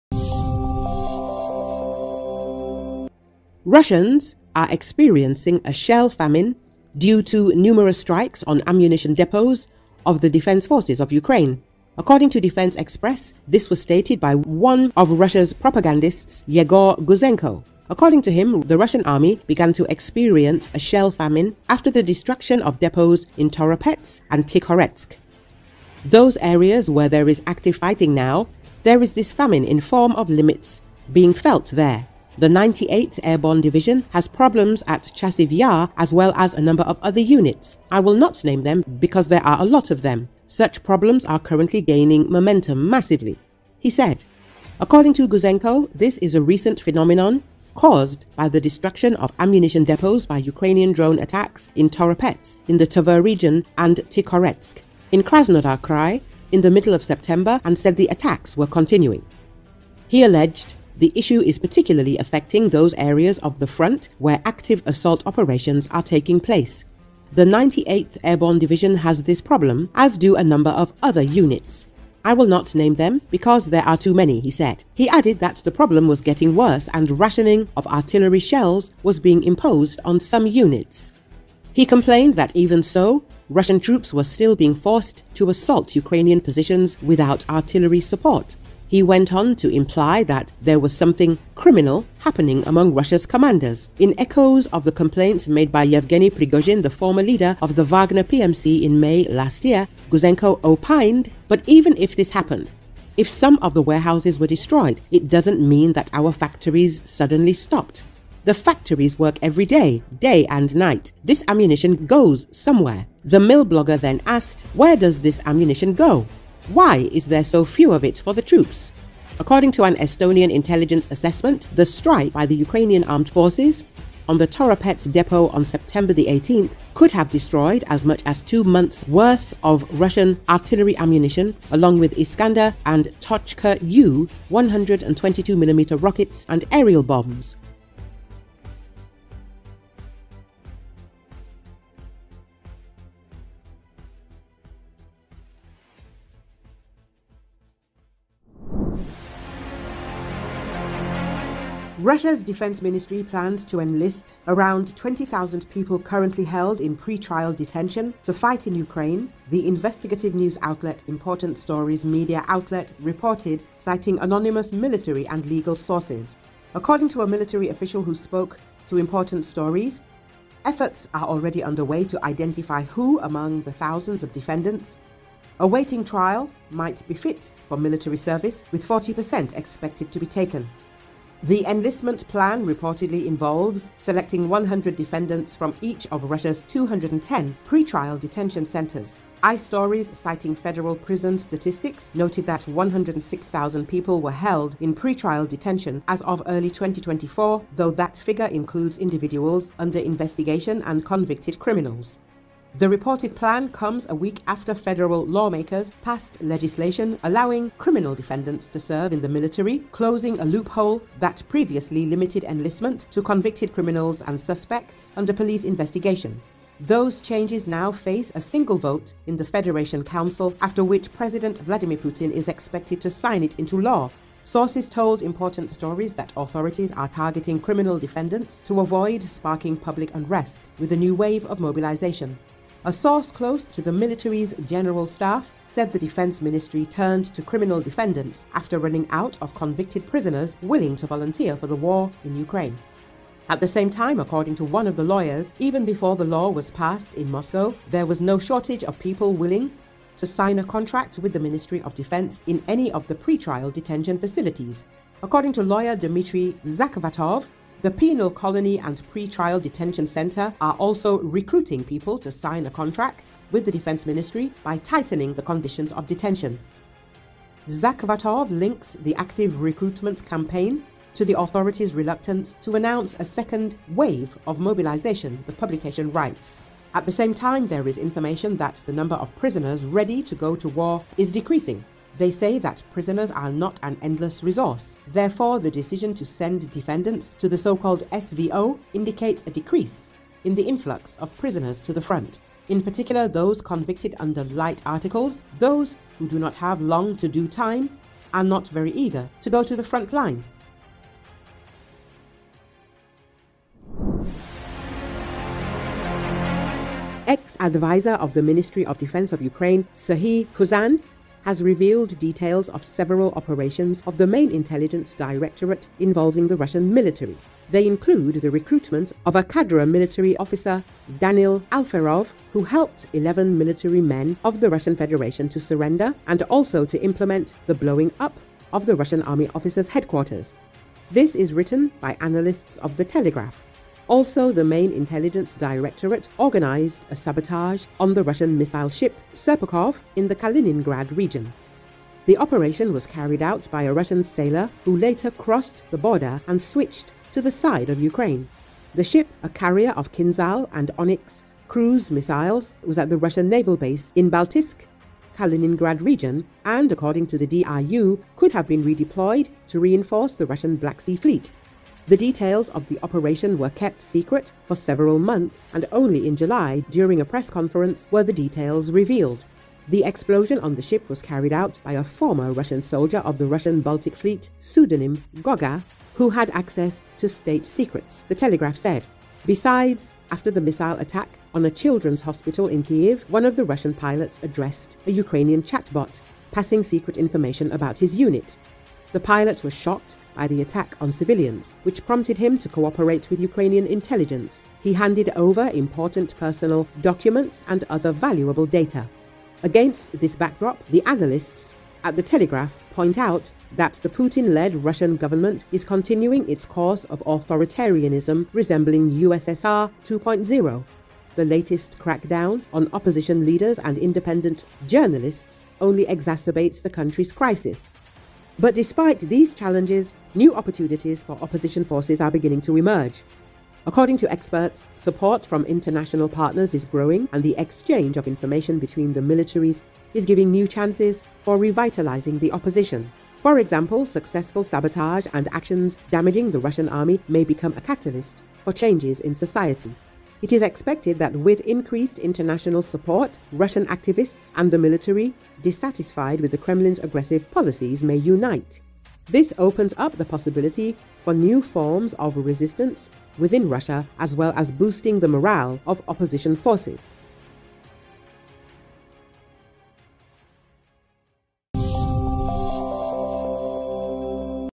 Russians complain of “shell famine” after Ukrainian strikes on ammunition depots
Russians are experiencing a “shell famine” due to numerous strikes on ammunition depots of the Defense Forces of Ukraine. According to Defense Express, this was stated by one of Russian propagandists Yegor Guzenko. According to him, Russian army began to experience a shell famine after the destruction of depots in Toropets and Tikhoretsk.

“Those areas where there is active fighting now, there is this famine in form of limits being felt there. The 98th Airborne Division has problems at Chasiv Yar as well as a number of other units. I will not name them because there are a lot of them. Such problems are currently gaining momentum massively,” the he said.

According to Guzenko, this is a recent phenomenon caused by the destruction of ammunition depots by Ukrainian drone attacks in Toropets in the Tver region and Tikhoretsk. in Krasnodar Krai in the middle of September and said the attacks were continuing.

He alleged the issue is particularly affecting those areas of the front where “active assault operations” are taking place. “The 98th Airborne Division has this problem, as do a number of other units. I will not name them, because there are too many,” he said. He added that the problem was getting worse and rationing of artillery shells was being imposed on some units.

He complained that, even so, Russian troops were still being forced to assault Ukrainian positions without artillery support.  He went on to imply that there was something “criminal” happening among Russia’s commanders. In echoes of the complaints made by Yevgeny Prigozhin, the former leader of the Wagner PMC in May last year, Guzenko opined:

“But even if this happened, if some of the warehouses were destroyed, it doesn’t mean that our factories suddenly stopped. The factories work every day – day and night. This ammunition goes somewhere.” The milblogger then asked, “Where does this ammunition go? Why is there so few of it for the troops?”. According to an Estonian intelligence assessment the strike by the Ukrainian Armed Forces on the Toropets depot on Sept. 18, could have destroyed as much as two months’ worth of Russian artillery ammunition along with Iskander and Tochka-U, 122mm rockets and aerial bombs.